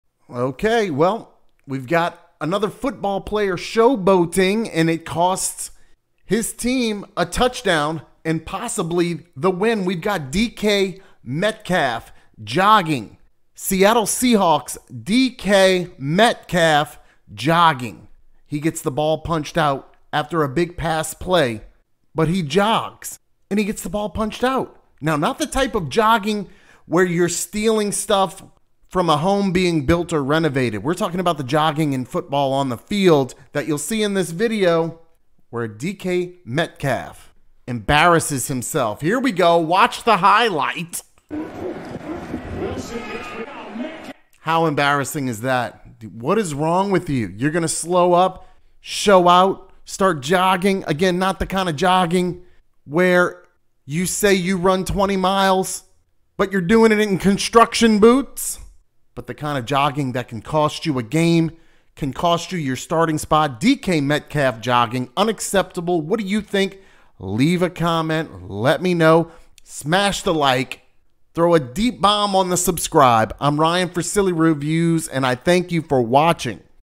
DK Metcalf Jogging - Seahawks Receiver Gets Ball Punched Out For Fumble
Another diva wide receiver out there showboating costs his team big time!
DK Metcalf jogging has Seahawks fans up in arms after what was a deep pass and a sure touchdown turns into a turnover.